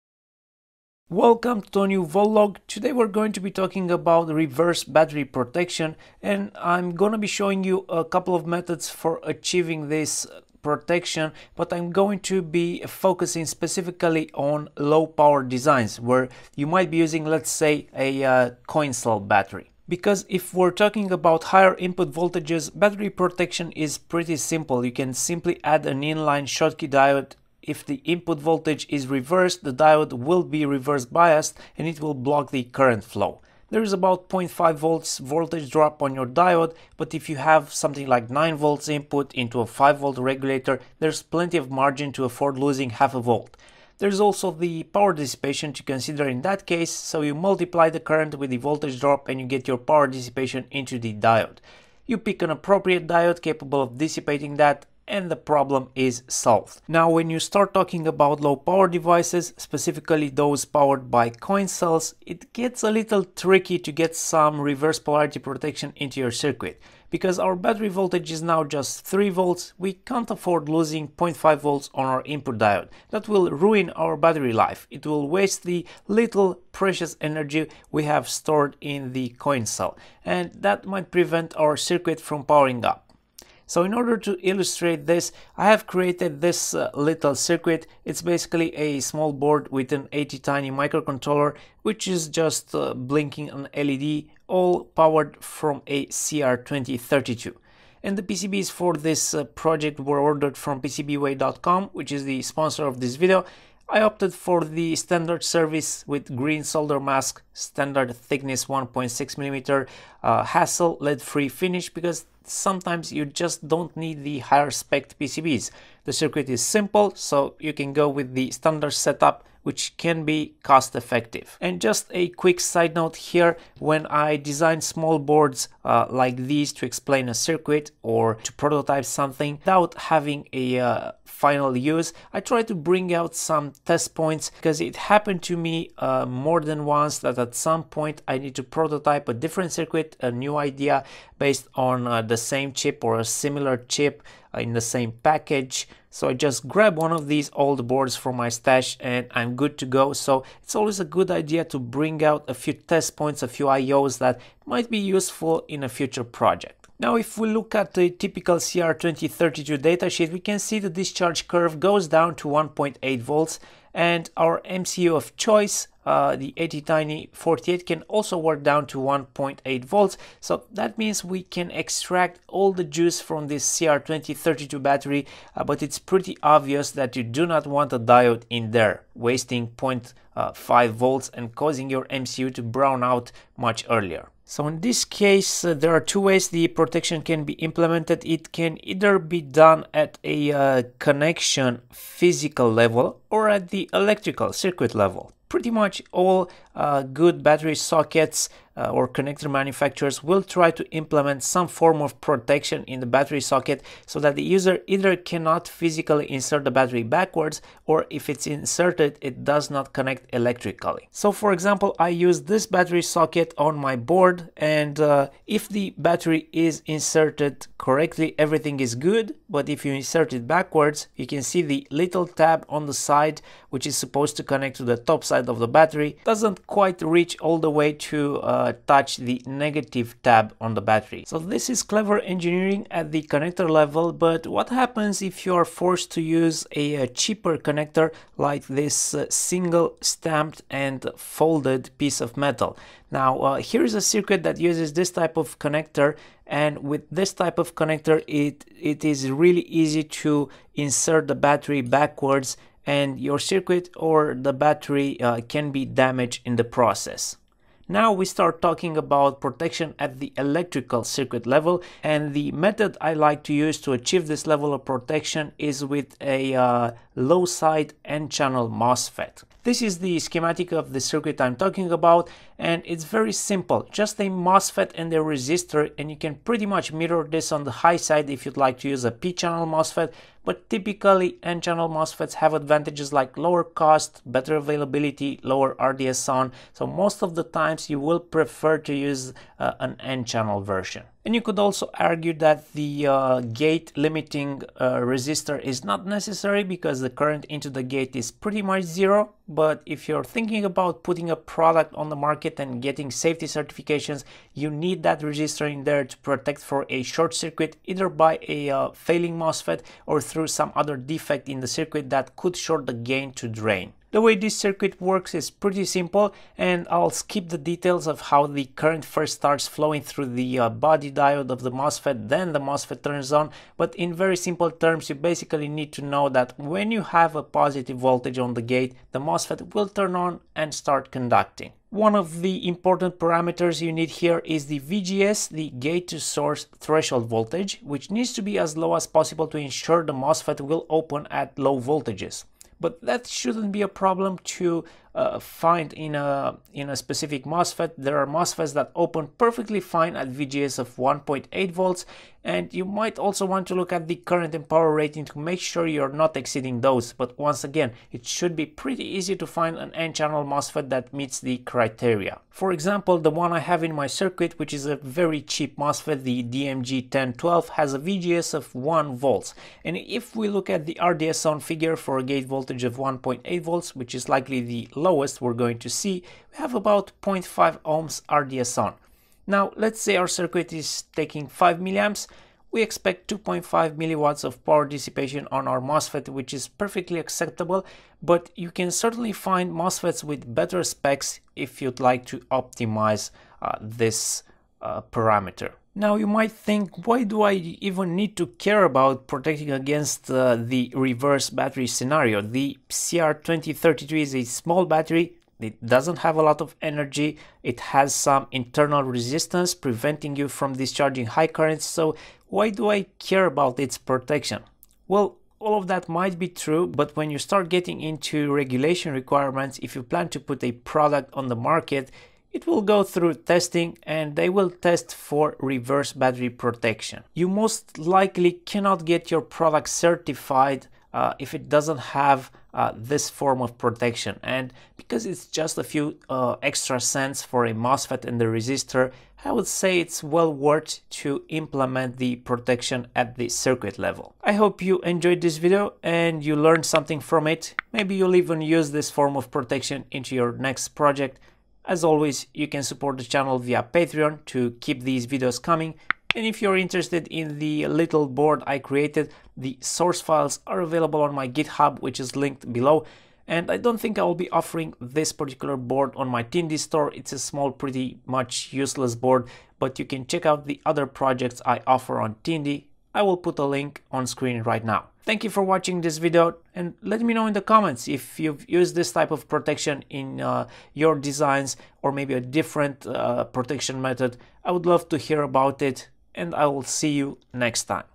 Reverse Battery Protection With Mosfet (no voltage drop) - Voltlog #353
High quality PCB Services In this video I’m showing you a simple circuit to achieve reverse battery protection in low power devices. Everyone knows the classic solution of using an in-line diode but that has one major drawback: the voltage drop on the diode. If your circuit is powered by a CR2032 you can’t afford losing 0.5V on the diode. The circuit I’m showing uses a mosfet to achieve the protection feature but minimizes the voltage drop and the losses because a mosfet will have a very low internal resistance when conducting.